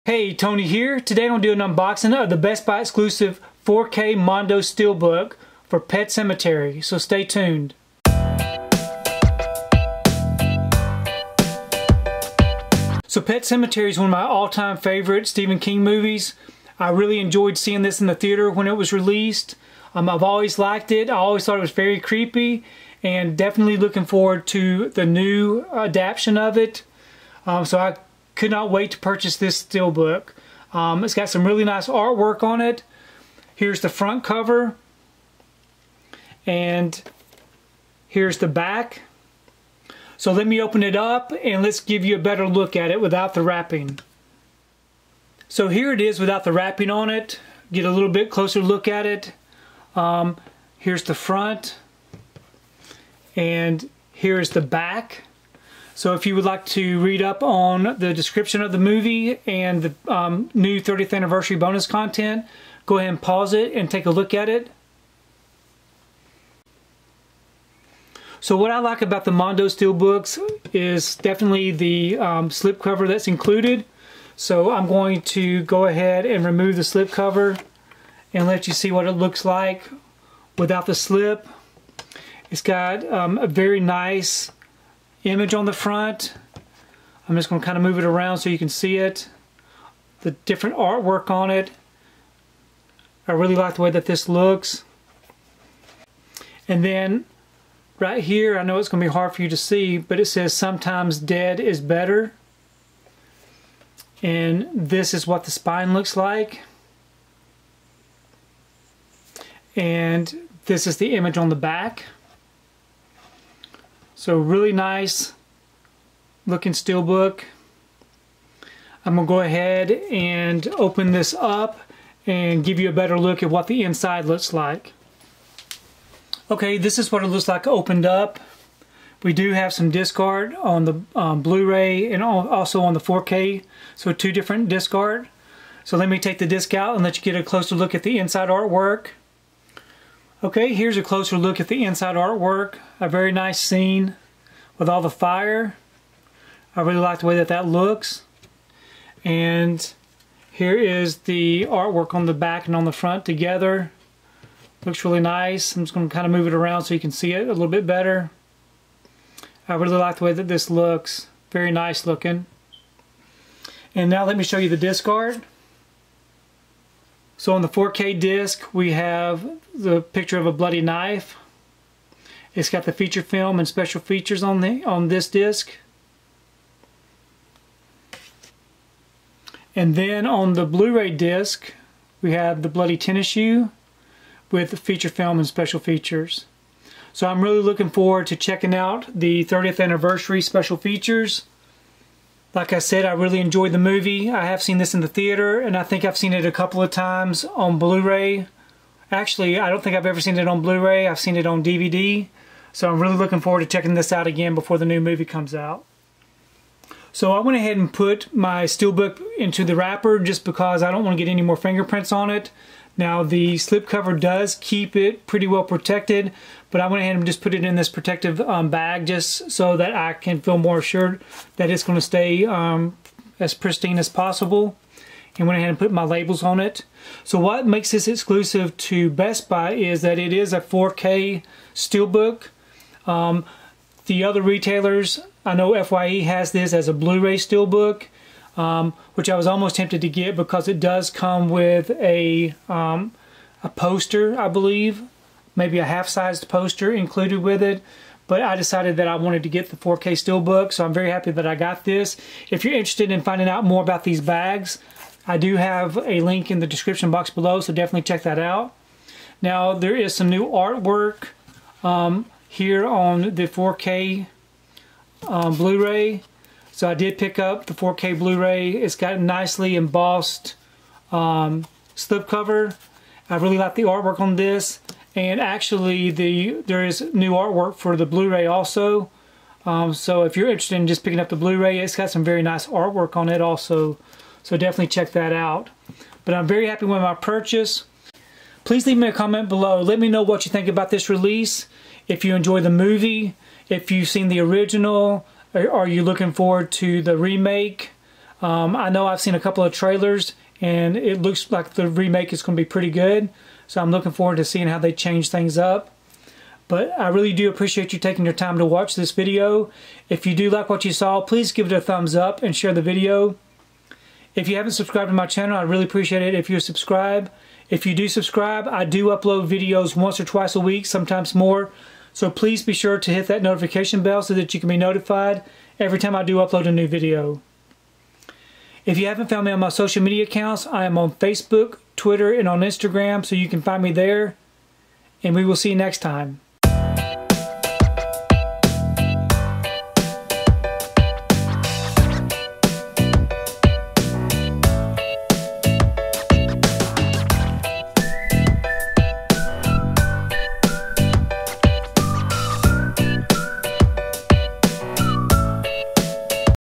Pet Sematary 4K Ultra HD Best Buy Exclusive Collectible Mondo SteelBook Unboxing
Check out my Pet Sematary 4K Ultra HD Best Buy Exclusive Collectible Mondo SteelBook Unboxing!

► Purchase your own Steelbook